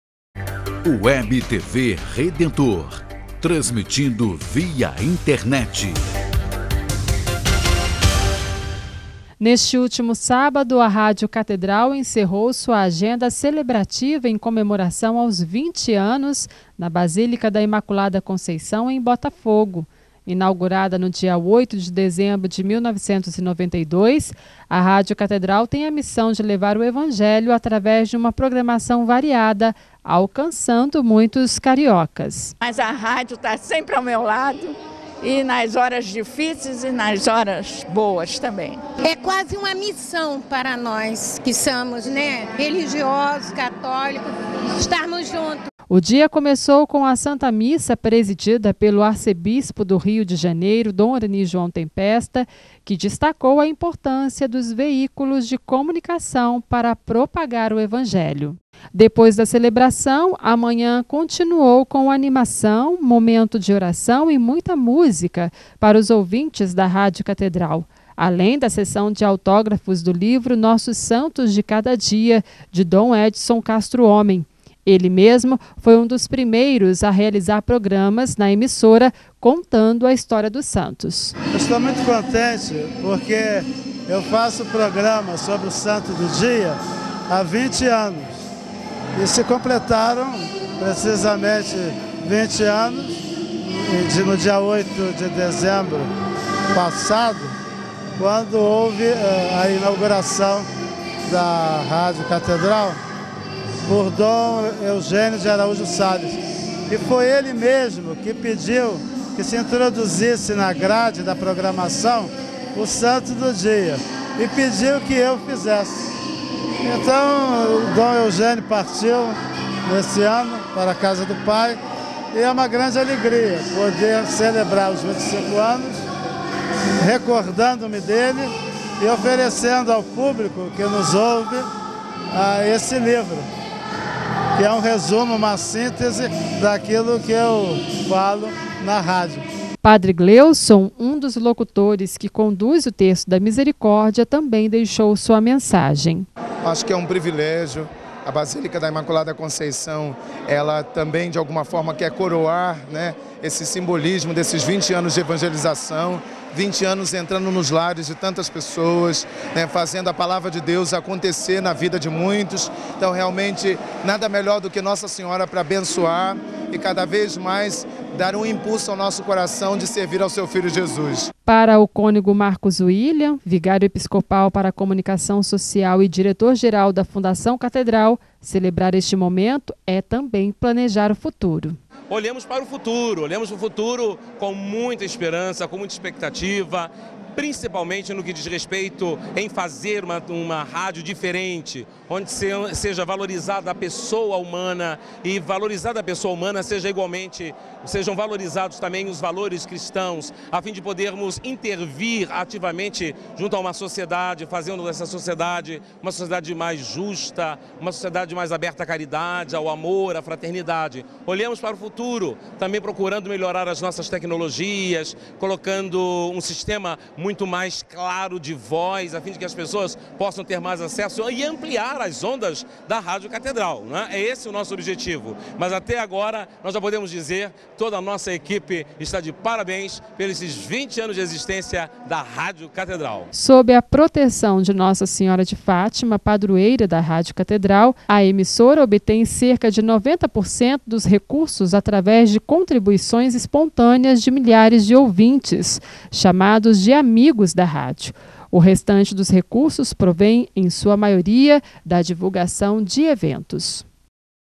Radio Catedral 20 Anos de Evangelização
Emissora Oficial da Arquidiocese do Rio de Janeiro, a Rádio Catedral, celebra 20 anos de história. evangelização através de uma programação variada, com: música, entretenimento, formação catequética e doutrinal.